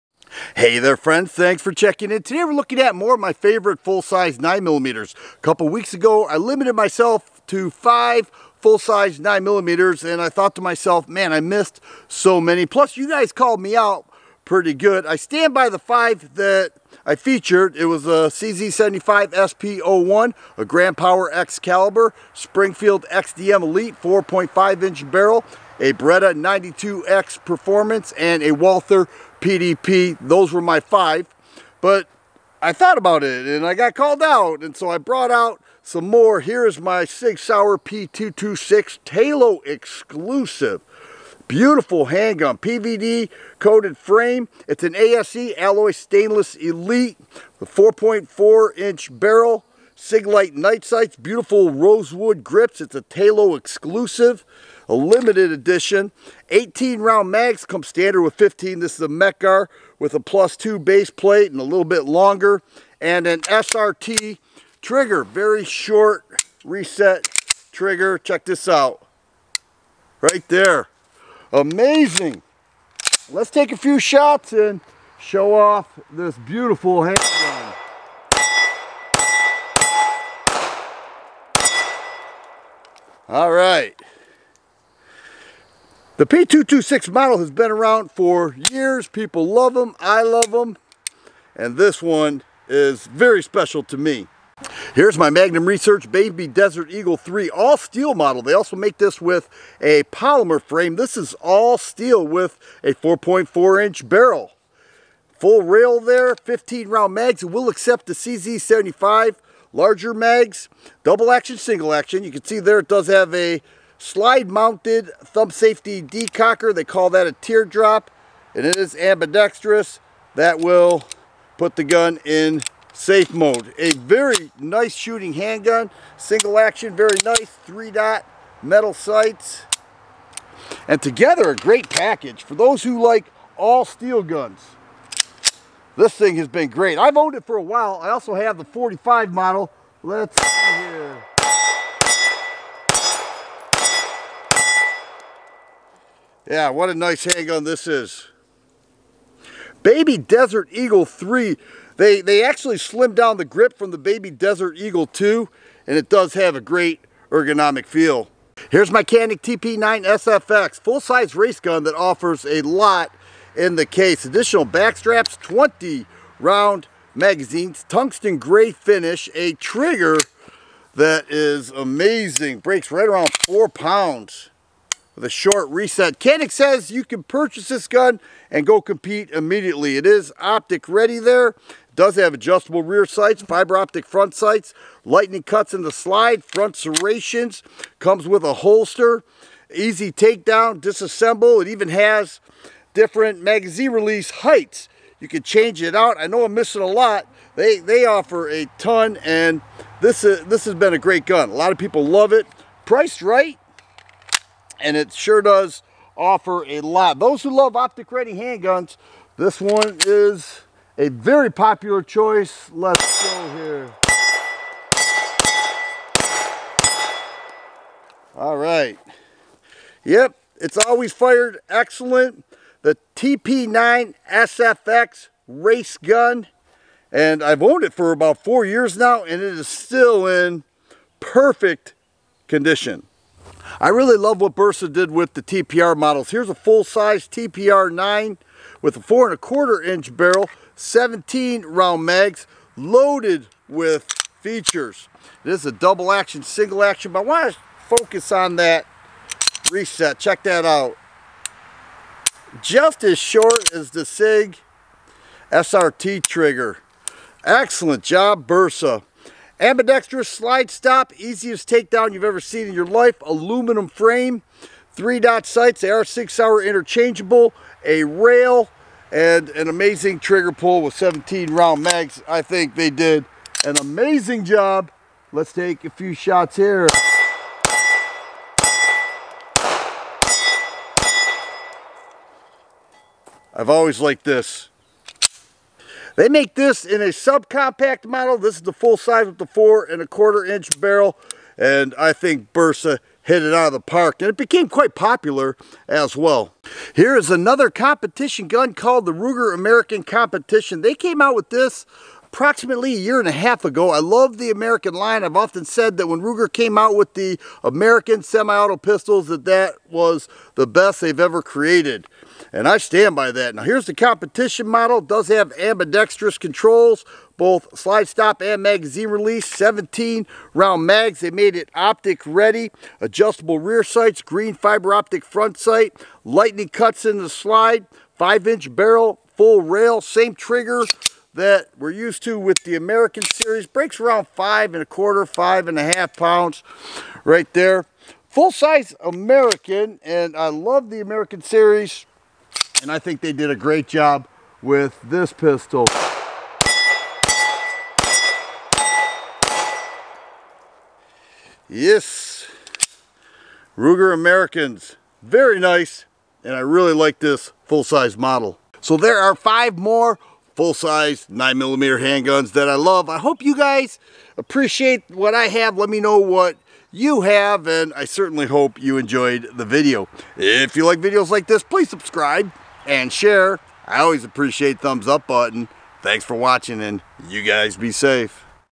Favorite Full-Size 9mm's (Part 2) - TheFirearmGuy
Here are my favorite full-size 9mm's part 2. I had to make a part 2 video featuring more of my favorite full-size 9mm handguns. Each of these 9mm handguns are special in their own way. They are showcased in no particular order. Let me which of these you like along with anything you feel I missed.